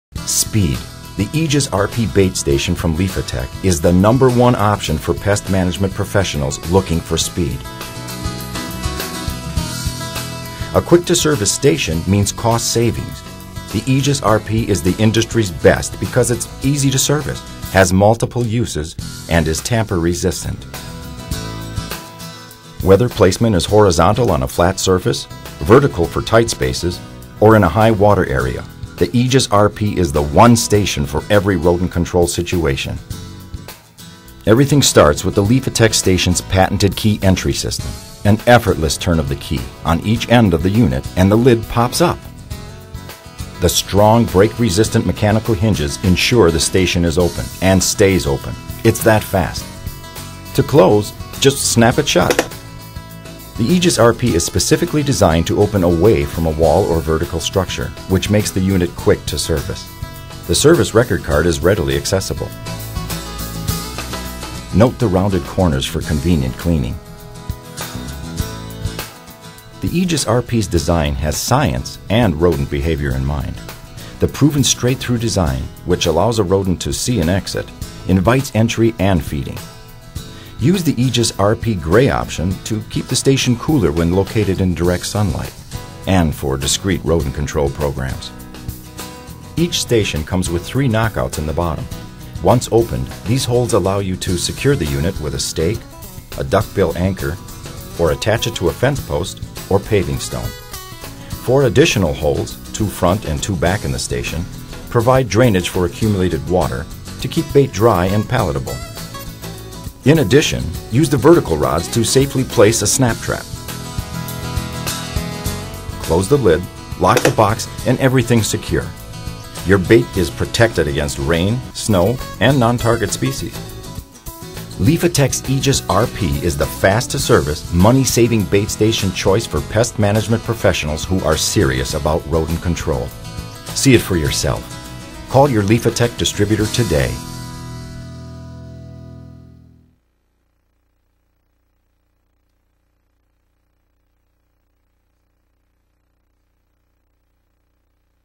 Aegis-RP Video
Are you looking for a quick-to-service bait station that is actually easy-to-open? Want a way to easily and safely secure a snap trap? What about an advanced technical design created to attract rodents? How about flexible mounting options for tight spaces? Then you're looking for the Aegis-RP from Liphatech, the leader in rodent-control technology.